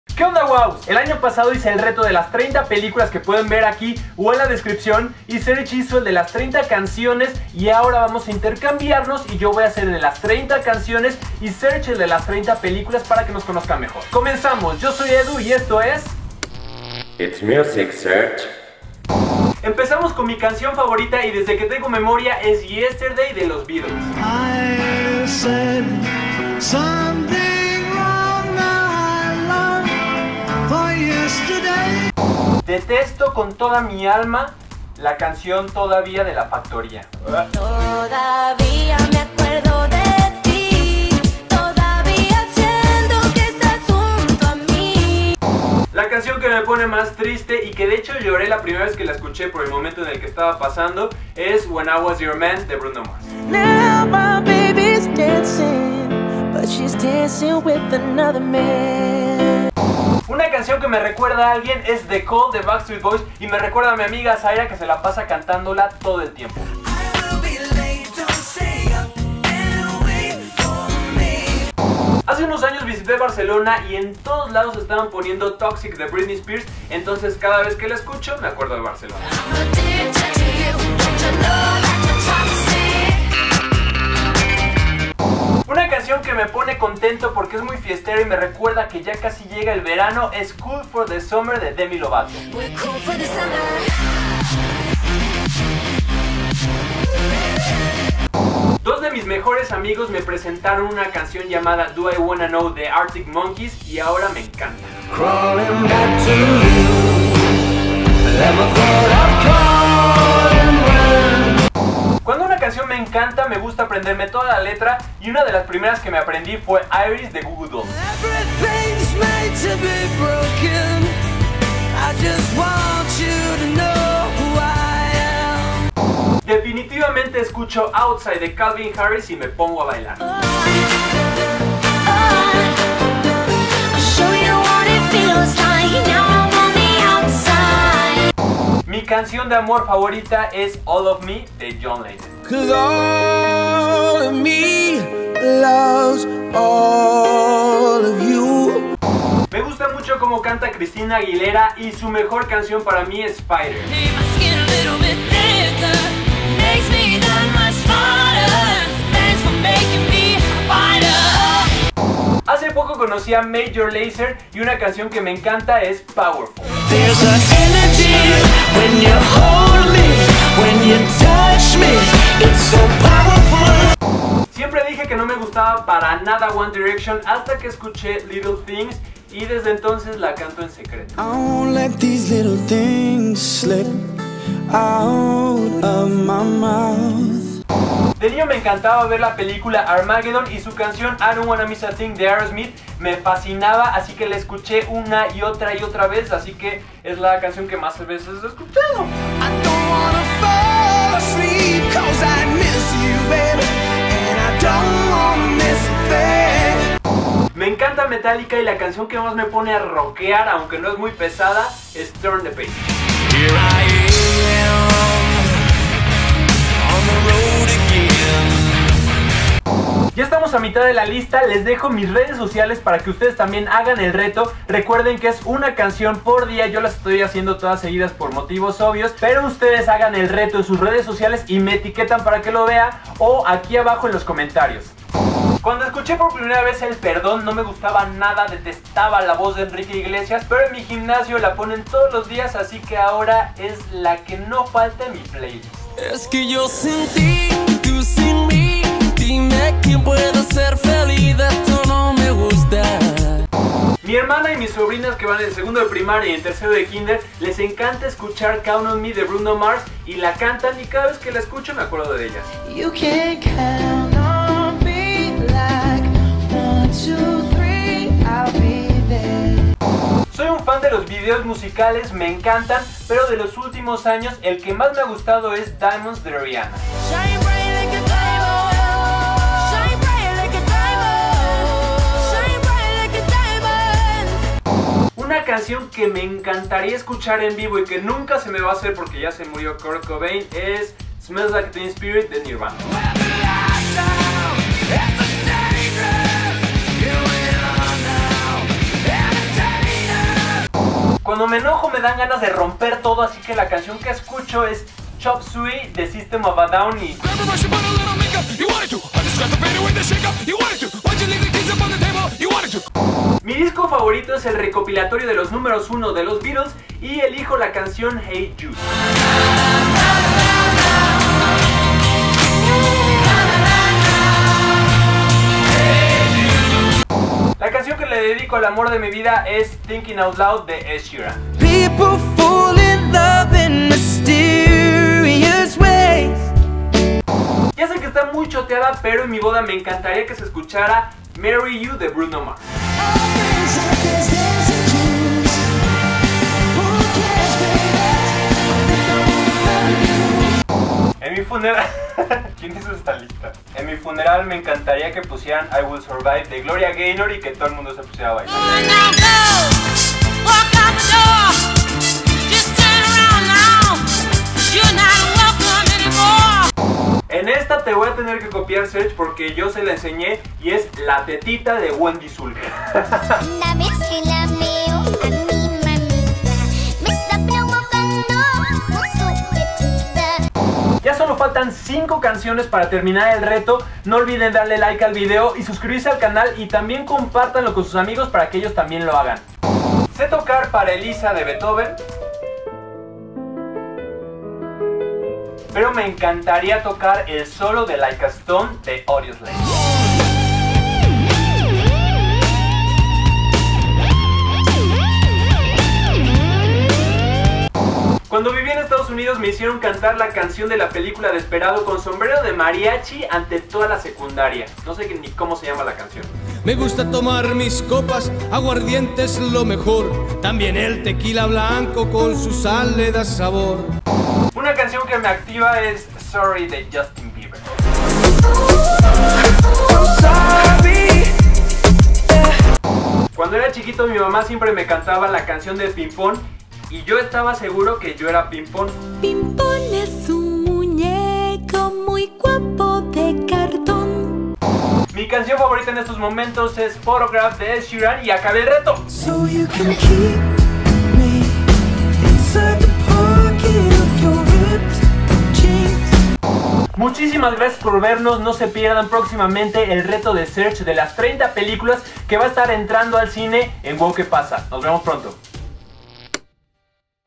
RETO 30 CANCIONES | DEMASIADA INFORMACIÓN | TAG 30 COSAS SOBRE MÍ | EDU WOW QUE PASA
¡Hoy hago el Reto 30 Canciones (Demasiada Información) para que me conozcan un poco más! Consiste en responder 30 cosas sobre mí con canciones. Se suele hacer en 30 días, pero yo lo voy a hacer todo seguido. ¡Les reto a que lo hagan! Más abajo les dejo todas las preguntas.

RETO 30 DÍAS, 30 CANCIONES (TAG 30 COSAS SOBRE MÍ):
día 01 - tu canción favorita 0:19
día 02 - la canción que más odias 0:31
día 03 - una canción que te pone triste 0:45
día 04 - una canción que te recuerda a alguien 1:00
día 05 - una canción que te recuerda un lugar 1:12
día 06 - una canción que te pone contento 1:25
día 07 - una canción que descubriste gracias a tu mejor amig@ 1:38
día 08 - una canción cuyas letras te sabes perfectamente 1:52
día 09 - tu canción favorita para bailar 2:06
día 10 - tu canción de amor favorita 2:18
día 11 - una canción de tu banda o cantante favorito 2:29
día 12 - una canción de una banda o cantante que acabas de descubrir 2:42
día 13 - una canción que te gusta en secreto 2:54
día 14 - la canción que más veces has escuchado en tu vida 3:08
día 15 - tu canción rock favorita 3:27
día 16 - una canción que no soportabas y ahora te encanta 3:55
día 17 - una canción que te hace pensar en alguien de tu familia 4:14
día 18 - la canción con el mejor video 4:30
día 19 - una canción que te encantaría escuchar en vivo 4:45
día 20 - una canción que te gusta escuchar cuando estás enojado 5:01
día 21 - una canción de tu disco favorito 5:13
día 22 - la canción que le dedicas al amor de tu vida 5:27
día 23 - una canción que quieres para tu boda 5:37
día 24 - una canción que quieres para tu funeral 5:51
día 25 - una canción que te hace reír 6:07
día 26 - una canción que puedes tocar con algún instrumento (o te gustaría) 6:30
día 27 - una canción que has cantado o te gustaría con público 6:48
día 28 - una canción que te activa 7:04
día 29 - una canción de tu infancia 7:13
día 30 - tu canción favorita en este momento 7:25

Too Much Information | Wow qué pasa | WQP! 2016 | Edu Rocha